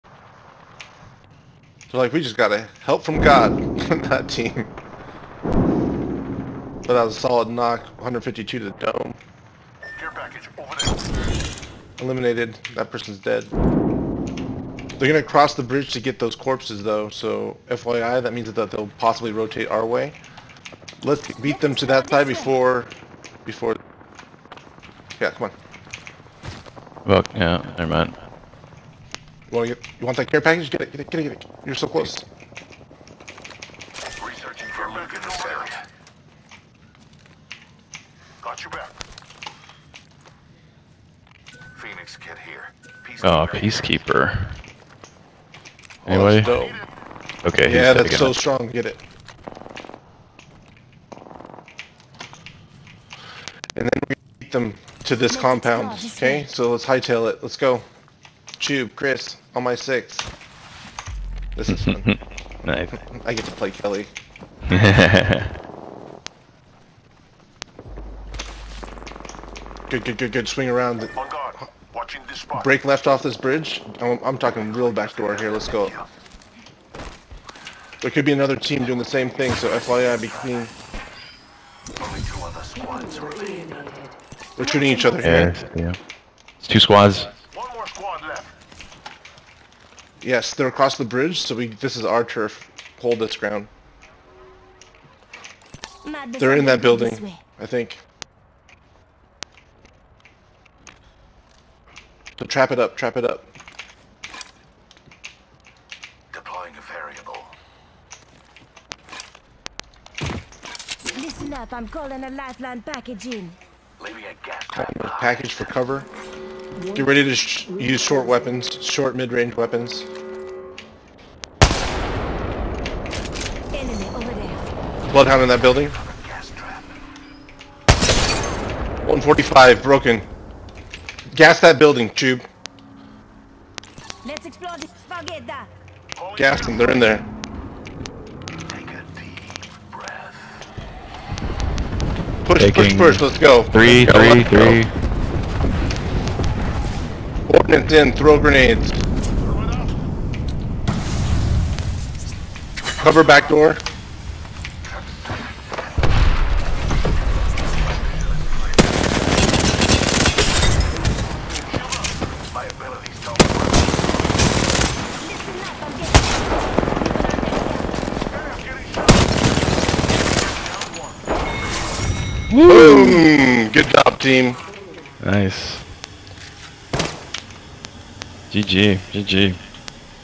Sunday Morning Win with Chuoob (PART 2)
Right before this clip opens I head shot an octane running across the bridge for 152 and dropped him. The recording missed by 5-10 seconds... doh!